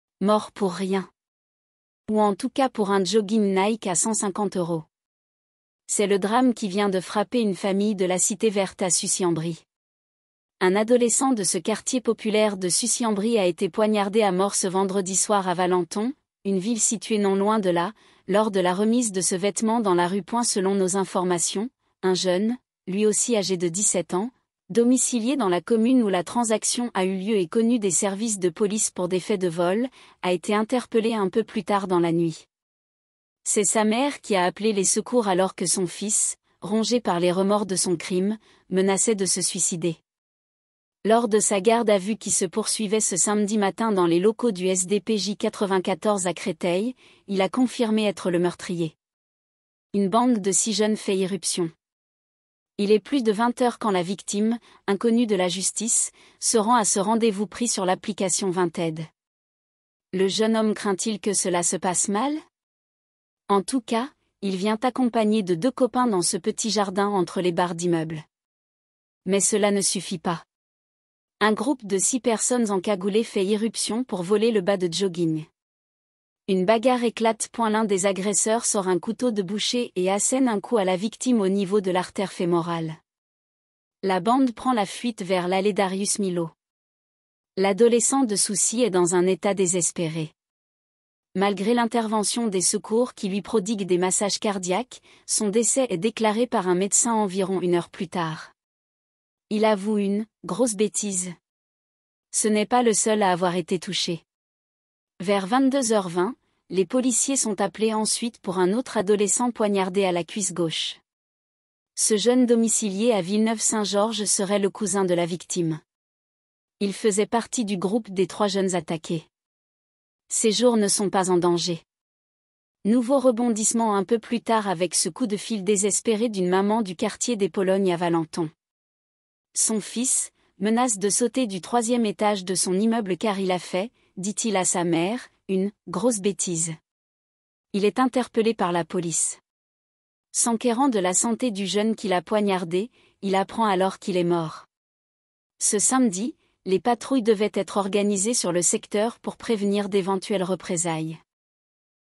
l’adolescent de 17 ans aurait été poignardé à mort pour un bas de jogging à 150 euros
Mort pour rien. Ou en tout cas pour un jogging Nike à 150 euros. C’est le drame qui vient de frapper une famille de la Cité Verte à Sucy-en-Brie. Un adolescent de ce quartier populaire de Sucy-en-Brie a été poignardé à mort ce vendredi soir à Valenton, une ville située non loin de là, lors de la remise de ce vêtement dans la rue.Selon nos informations, un jeune, lui aussi âgé de 17 ans, domicilié dans la commune où la transaction a eu lieu et connu des services de police pour des faits de vols, a été interpellé un peu plus tard dans la nuit. C’est sa mère qui a appelé les secours alors que son fils, rongé par les remords de son crime, menaçait de se suicider. Lors de sa garde à vue qui se poursuivait ce samedi matin dans les locaux du SDPJ 94 à Créteil, il a confirmé être le meurtrier.
Une bande de six jeunes fait irruption
Il est plus de 20 heures quand la victime, inconnue de la justice, se rend à ce rendez-vous pris sur l’application Vinted. Le jeune homme craint-il que cela se passe mal ? En tout cas, il vient accompagné de deux copains dans ce petit jardin entre les barres d’immeuble. Mais cela ne suffit pas. Un groupe de six personnes encagoulées fait irruption pour voler le bas de jogging. Une bagarre éclate.L’un des agresseurs sort un couteau de boucher et assène un coup à la victime au niveau de l’artère fémorale. La bande prend la fuite vers l’allée Darius-Milhaud. L’adolescent de Sucy est dans un état désespéré. Malgré l’intervention des secours qui lui prodiguent des massages cardiaques, son décès est déclaré par un médecin environ une heure plus tard.

Il avoue une « grosse bêtise »
Ce n’est pas le seul à avoir été touché. Vers 22h20, les policiers sont appelés ensuite pour un autre adolescent poignardé à la cuisse gauche. Ce jeune domicilié à Villeneuve-Saint-Georges serait le cousin de la victime. Il faisait partie du groupe des trois jeunes attaqués. Ses jours ne sont pas en danger.

Nouveau rebondissement un peu plus tard avec ce coup de fil désespéré d’une maman du quartier des Pologne à Valenton. Son fils, menace de sauter du troisième étage de son immeuble car il a fait, dit-il à sa mère, une « grosse bêtise ». Il est interpellé par la police. S’enquérant de la santé du jeune qu’il a poignardé, il apprend alors qu’il est mort.

Ce samedi, les patrouilles devaient être organisées sur le secteur pour prévenir d’éventuelles représailles.